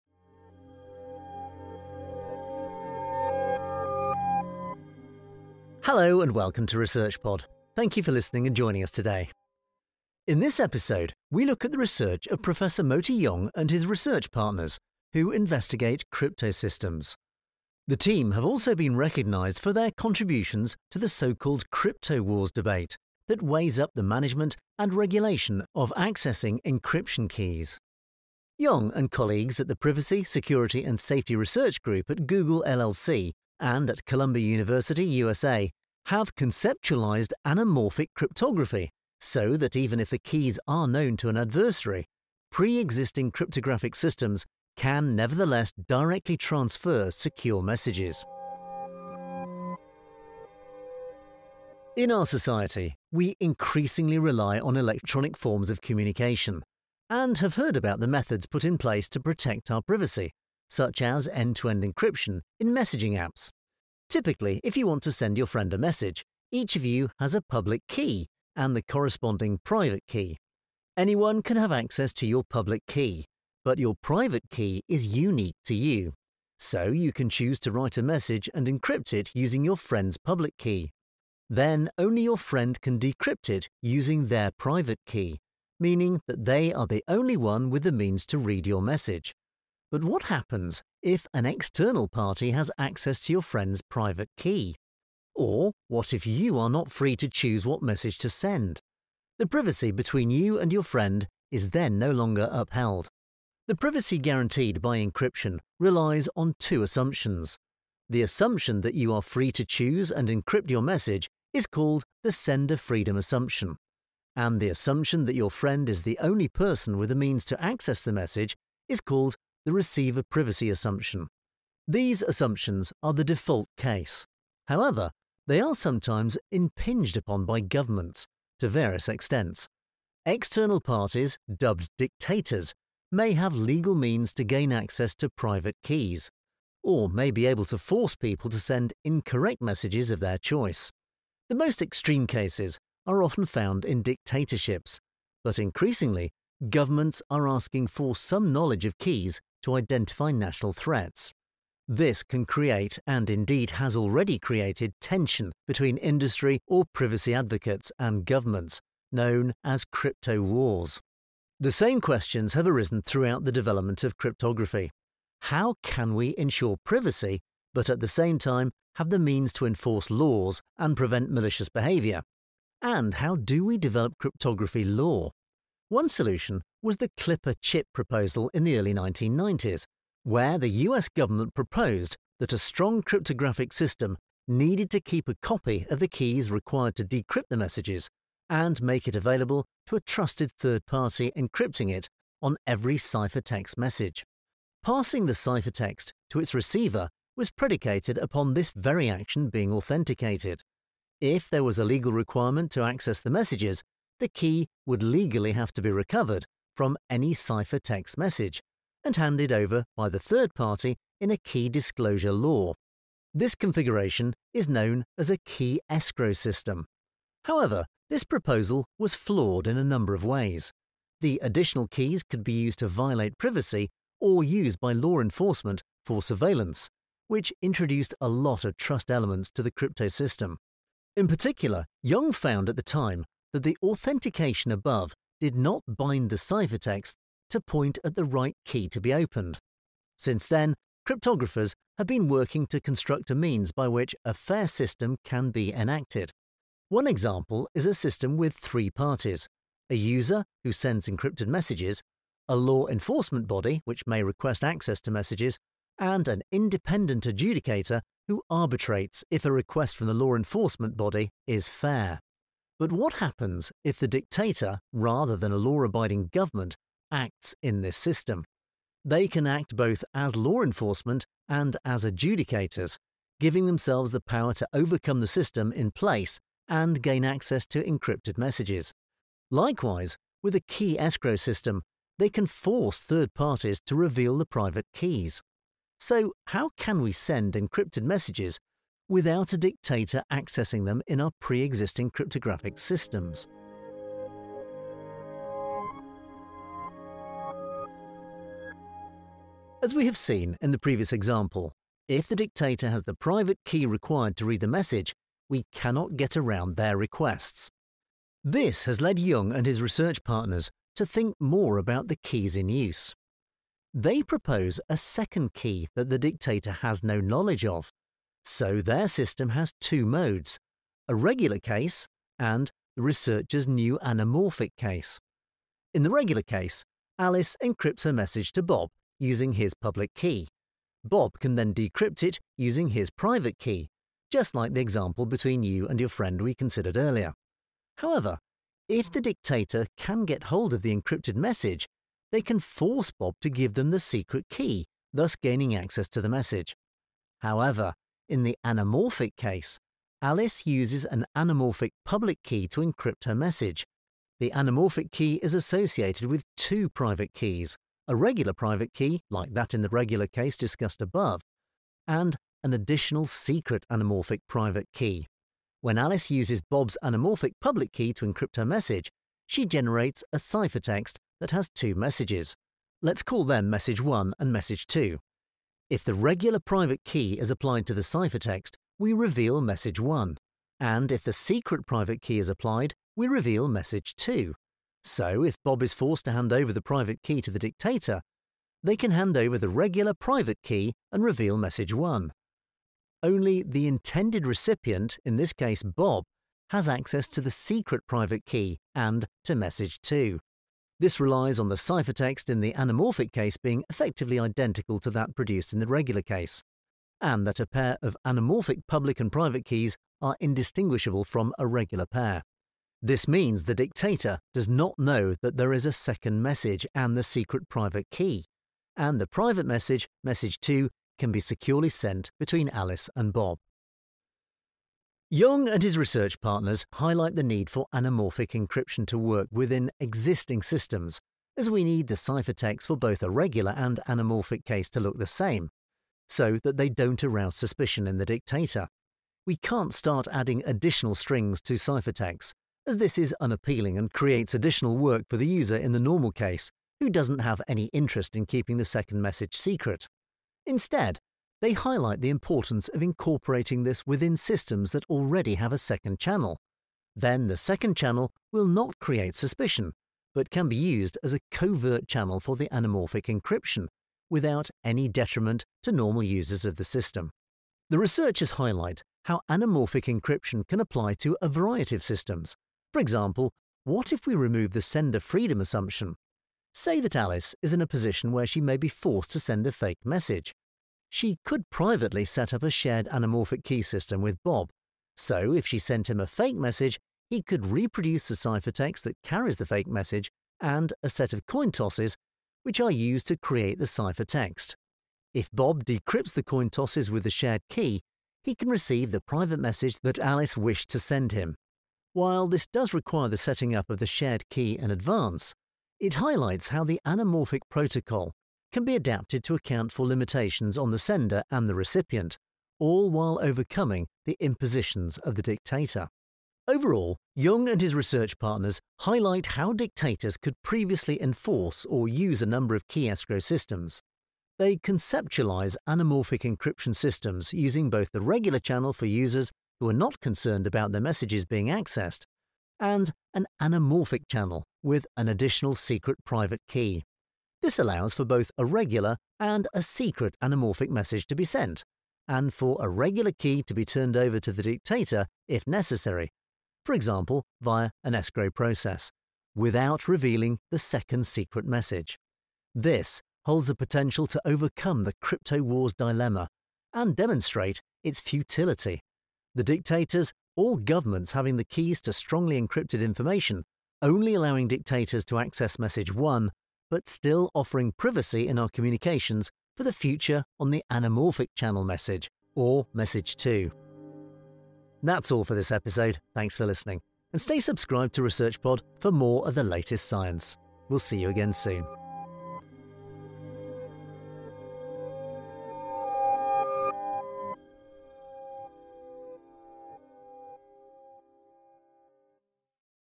How can we ensure private communication?
We increasingly rely on electronic communications across society. You may have heard of privacy protection methods for those messages, such as end-to-end encryption. However, can that protection be guaranteed against governments, industries or bad actors?

How can we ensure privacy, but at the same time have the means to enforce laws and prevent malicious behaviour, and how do we develop cryptography law?

Professor Moti Yung and colleagues at the Privacy, Security, and Safety Research Group at Google LLC and at Columbia University, USA, have conceptualised ‘anamorphic’ cryptography, and have also been recognised for their contributions to the so-called ‘Crypto Wars’ debate.